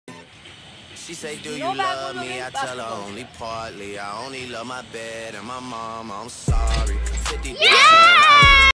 A Polar bear playing basketball..
Hey what's up guys, Mr hunt here!  Went to Antarctica with Ashish Chanchalani...  See what happened there...